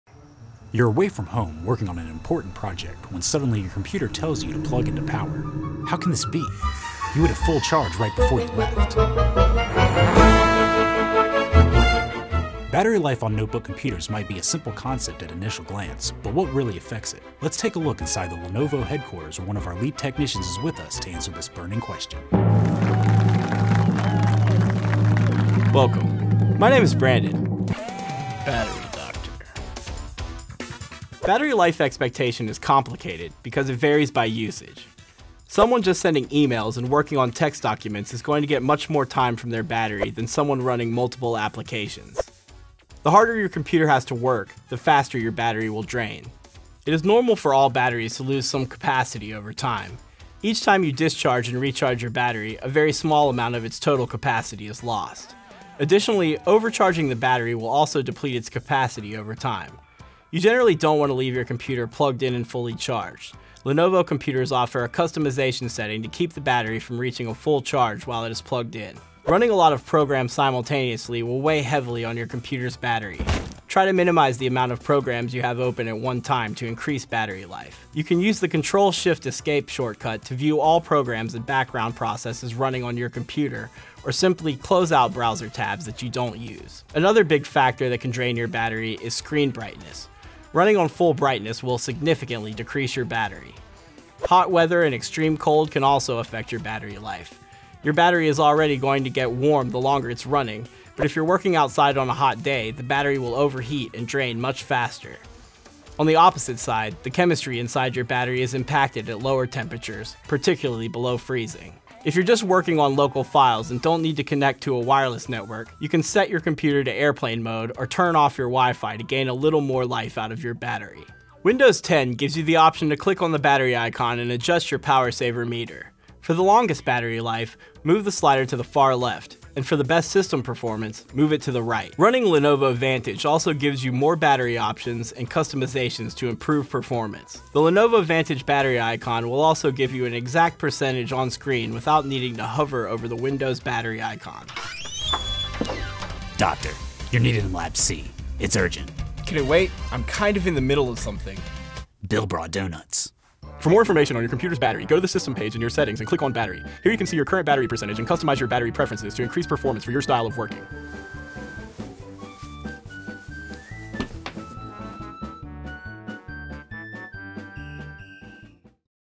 Battery Life and How it Affects You
Battery life is something that affects us on a daily basis.  Whether it's your phone, computer, or tablet we're constantly monitoring our battery percentage.  So what can you do to increase battery life when you're not able to charge your device?  We have one of our lead technicians with us to answer this question.